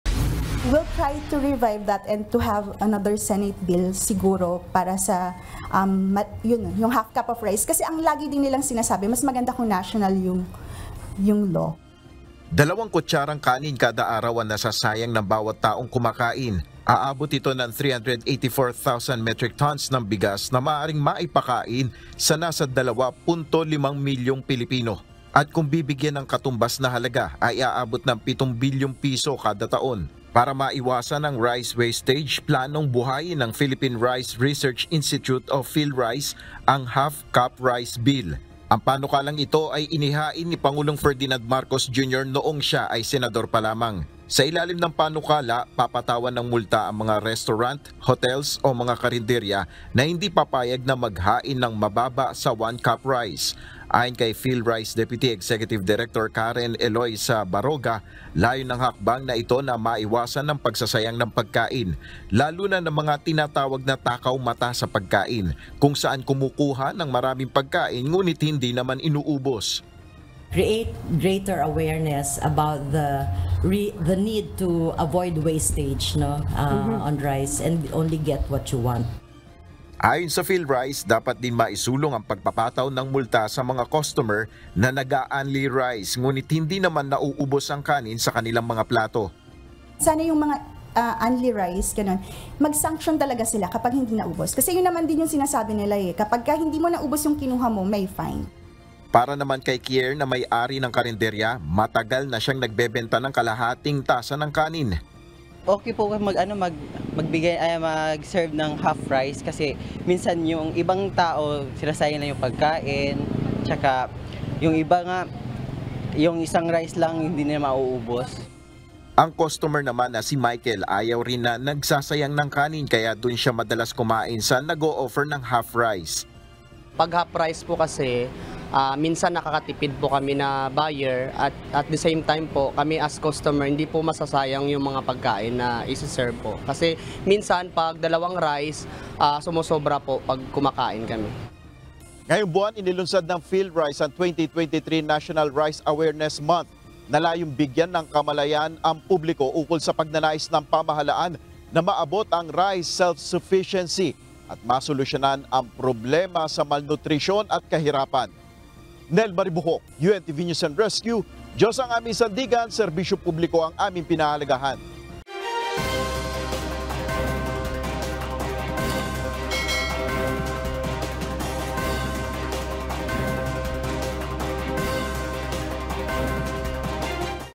Half-cup rice bill, planong buhayin ng PhilRice dahil sa nasasayang na kanin kada araw
Nais ng Philippine Rice Research Institute o PhilRice na buhayin ang Half-Cup Rice bill para maisulong ang pagtitipid sa kanin.

Ayon sa ahensya, maraming butil ang nasasayang kada araw dahil umano sa mga mahihilig mag-unli rice pero hindi naman inuubos ang kanin sa kanilang mga plato.